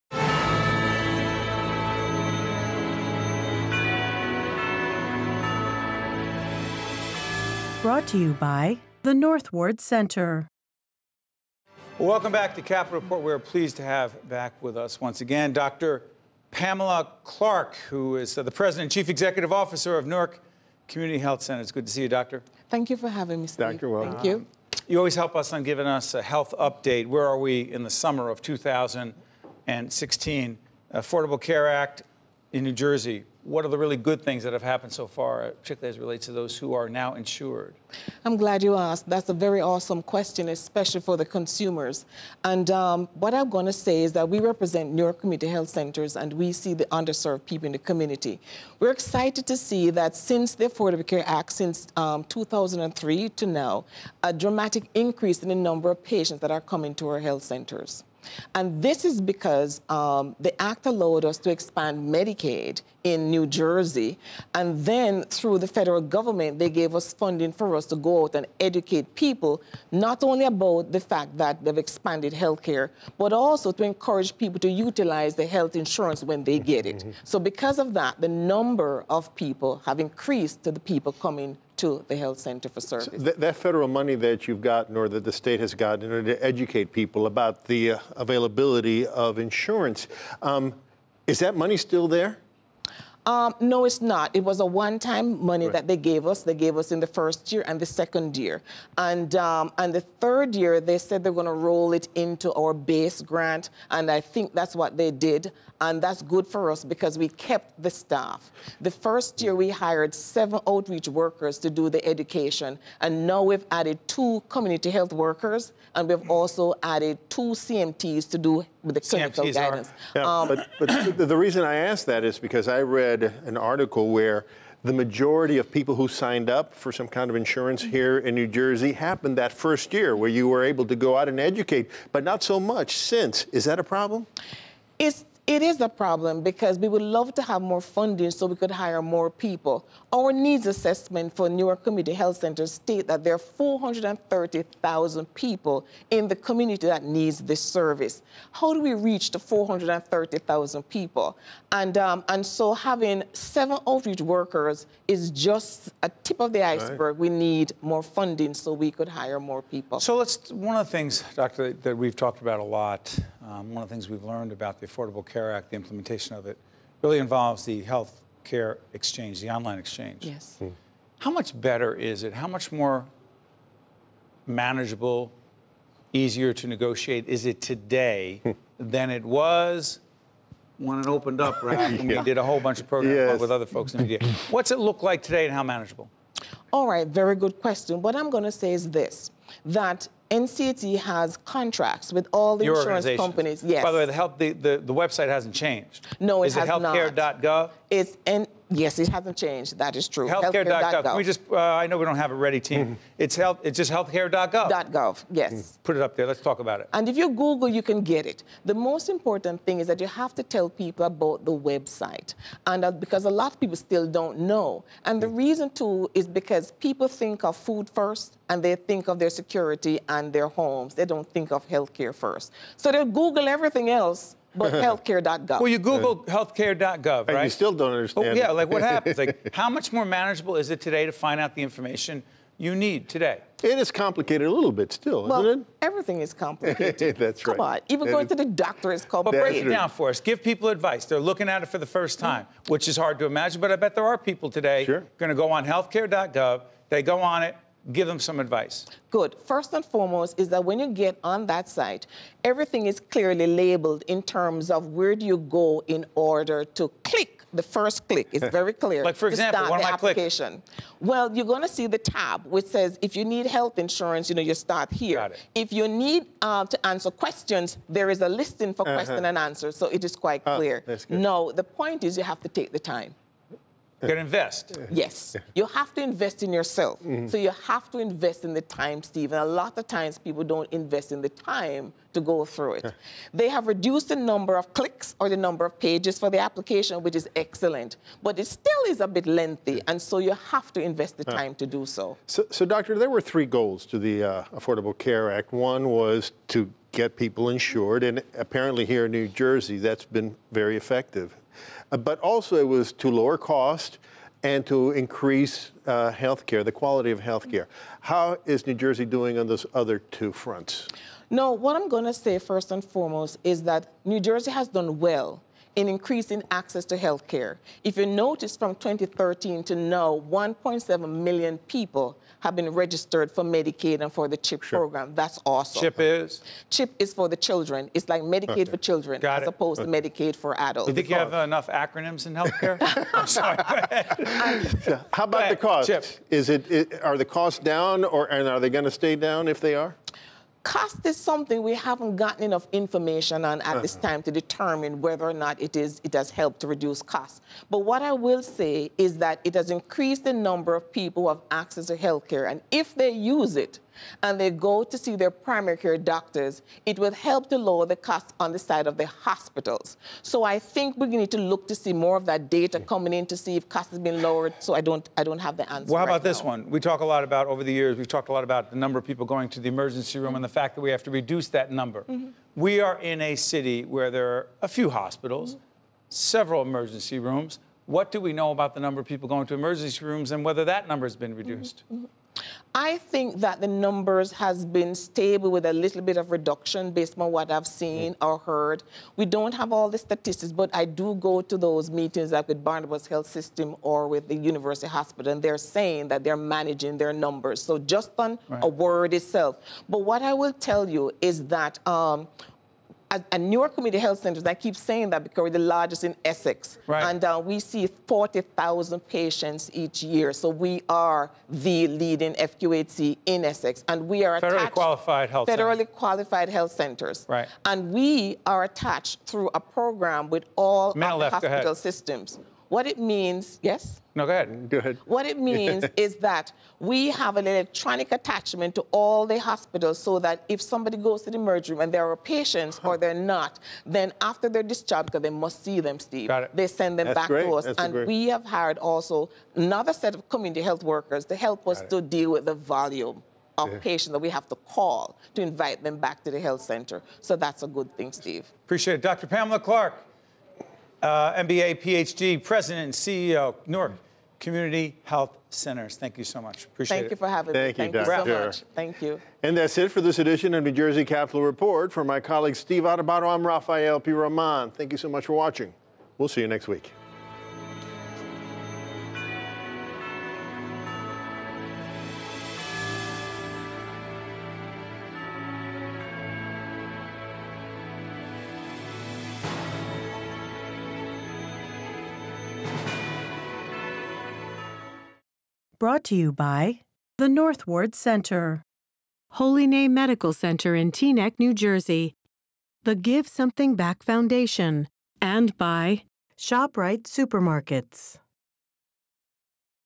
The Success of NJ's Children's Health Insurance Program
Pamela Clarke, Ph.D., President & CEO of Newark Community Health Center, Inc., talks about the positive impact of the Affordable Care Act in New Jersey and the success of individual enrollment into the Children’s Health Insurance Program. (Politicker NJ)